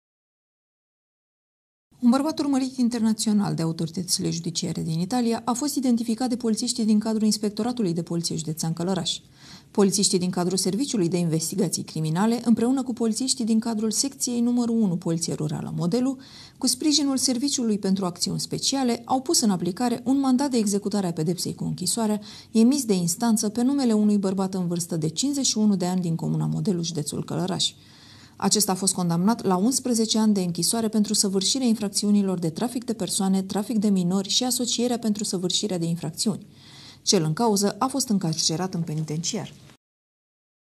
Știri Locale - Urmărit internațional, prins la Modelu
Polițiștii din cadrul Serviciului de Investigații Criminale împreună cu polițiști din cadrul Secției 1 Poliție Rurală Modelu, cu sprijinul Serviciului pentru Acțiuni Speciale, au pus în aplicare un mandat de executare a pedepsei cu închisoarea, emis de instanță, pe numele unui bărbat în vârstă de 51 de ani, din comuna Modelu, județul Călărași.